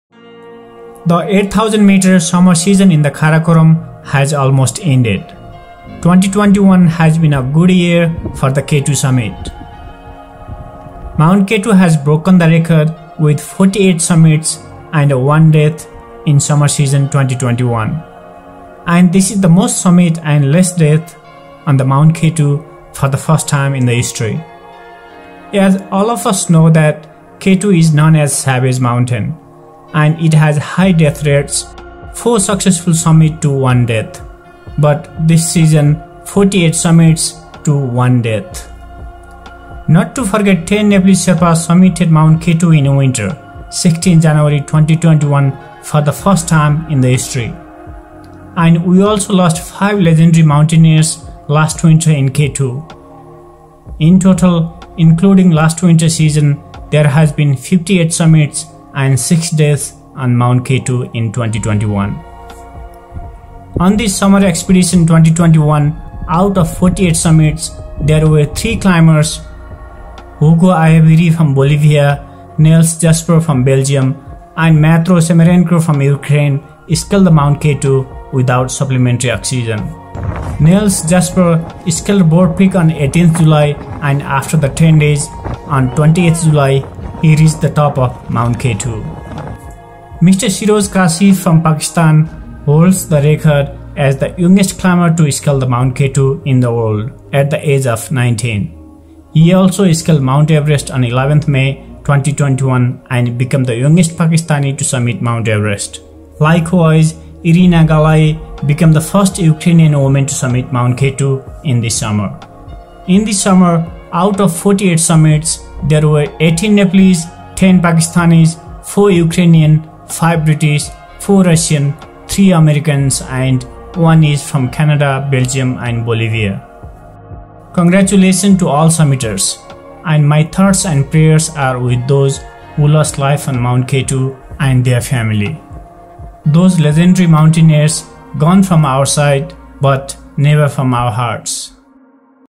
K2 2021 - 58 SUMMITS & 6 DEATHS
The 8,000m summer season in the Karakorum has almost ended. K2 2021SUMMER EXPEDITION has been a good season for the K2 summit, Mt K2 has broken the record with 48 summits and 1 death in summer season 2021, and this is the most summit and less death on MT K2 for the first time in history. As all of us know that K2 is known as Savage Mountain and it has high death rates, 4 successful summits to 1 death but this season 48 summits to 1 death.
Not to forget 10 Nepalese Sherpa summited Mt K2 in winter 16 Jan 2021 for the first time in history. And we also lost 5 legendary mountaineers last winter in K2. In total including last winter season, there have been 58 summits and 6 deaths on MT K2 in 2021.

On this summer expedition 2021 out of 48 summits, there were 3 climbers Hugo Ayaviri from bolivia, Neils Jasper from Belgium, and Dmytro Semerenko from Ukraine
Scaled the MT K2 without supplementary oxygen
Neils Jasper scaled Broad peak on 18th July and after 10 days on 28th July, he reached on top of MT K2.
Mr. Shehroze Kashif from Pakistan holds the record as the youngest climber to scale the MT K2 in the world at the age of 19.  He also scaled Mt Everest on 11 May 2021 and became the youngest Pakistani to summit Everest. And Irina Galay became the first Ukrainian woman to summit K2.
 Congratulations to all summiteers and my thoughts and prayers are with those who lost life on MT K2 and their family, those legendary mountaineers have gone from our sight, but never from our hearts.

This summer, a total of 48 summits, 18 Nepalese, 10 Pakistanis, 4 Ukrainian, 5 British, 4 Russian, 3 Americans, and 1 each from Canada, Belgium, and Bolivia.

Errors & Omissions will be accepted

Thanks and Namaste